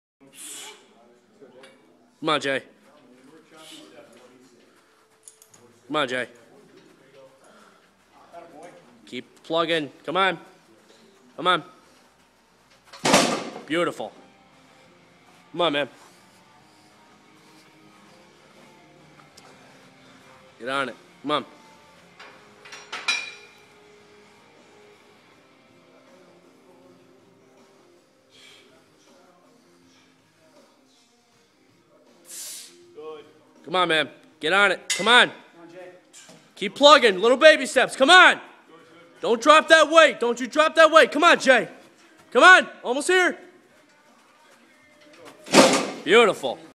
Jason Cholewa 600 pound yolk walk
600 pound yolk walk for 15 yards up and 15 yards back

@ Just Performance Fitness LLC